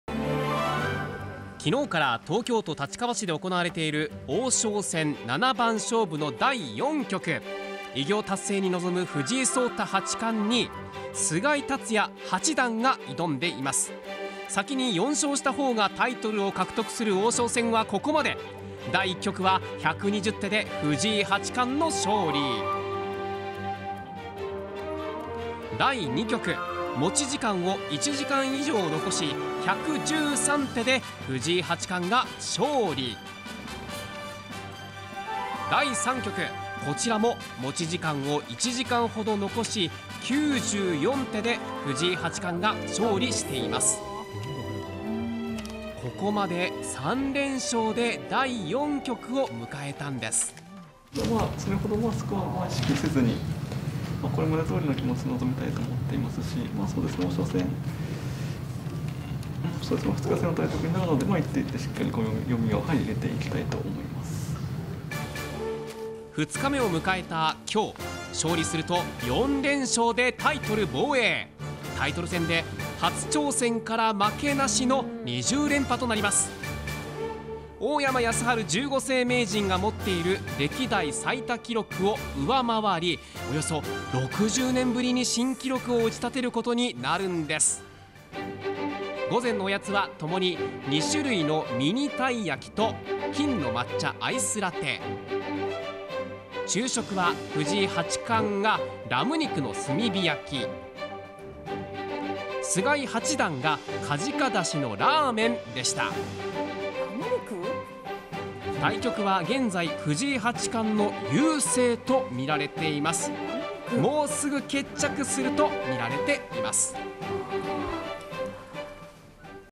【歴代単独１位の偉業へ】藤井八冠 勝てば“２０連覇” ８大タイトル「王将戦」
愛知県瀬戸市出身の藤井聡太八冠は、８日の対局に勝利すれば、タイトル戦、２０連覇の新記録を打ち立てることになります。【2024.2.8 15:50 放送】